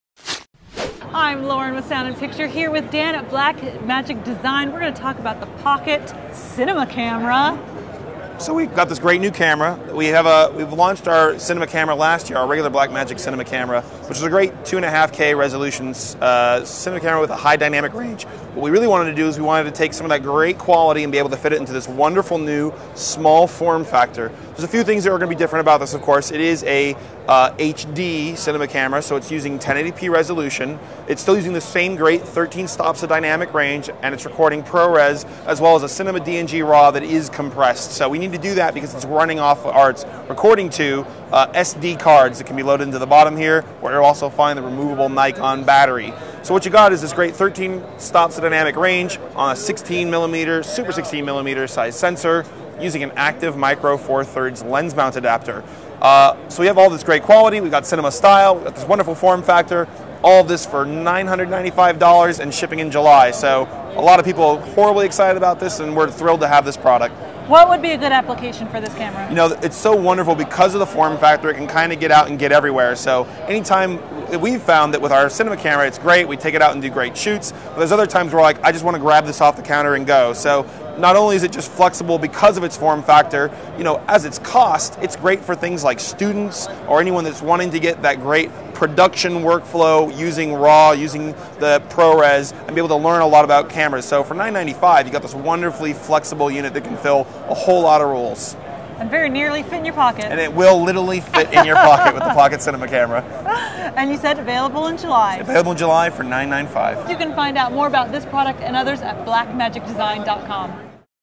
NAB Show 2013 - Blackmagic Design Pocket Cinema Camera
With a price tag under 1K, this radically small ProRes 422 (HQ) camera from Blackmagic can capture a pretty stellar image.